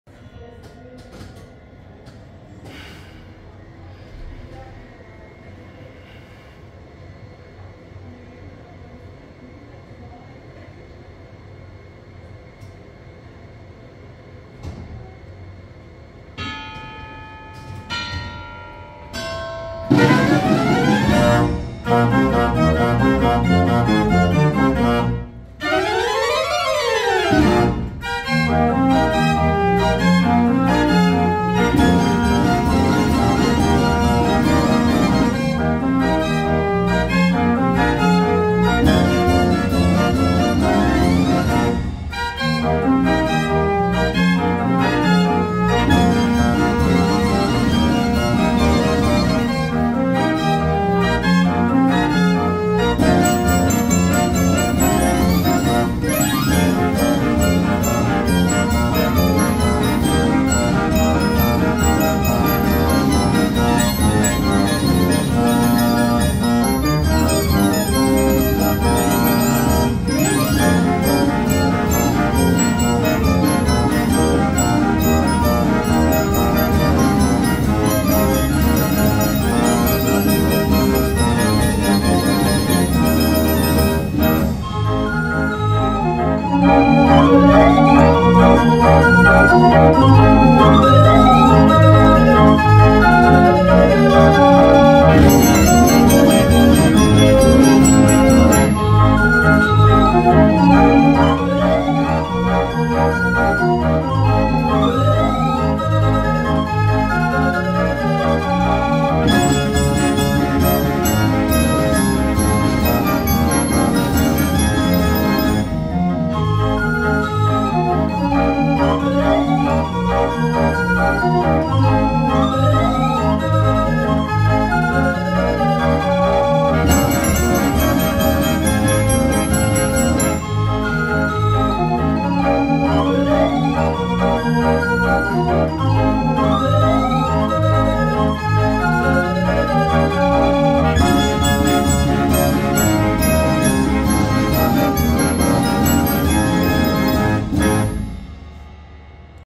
Karl Frei Jahrmarktsorgel Baujahr 1949. Musikstück unbekannt. Gespielt im Museum für Musikautomaten.
Heute machte ich mit einpaar Gästen ein Ausflug im Museum für Musikautomaten in Seewen Solothurn gehabt. Es war schön und spannend. Jedenfalls habe ich diesen wunderbaren Karl Frei Jahrmarktsorgel Baujahr 1949 aus Waldkirch Freiburg in Breisgau. Aufgenommen und ich gab mir sehr Mühe und falls ihr das Stück kennt und noch Fragen habt einfach wie immer in die Kommentare schreiben okay vielen herzlichen Dank und die restlichen Aufnahmen kommen versprochen. Liebe Grüße von mir.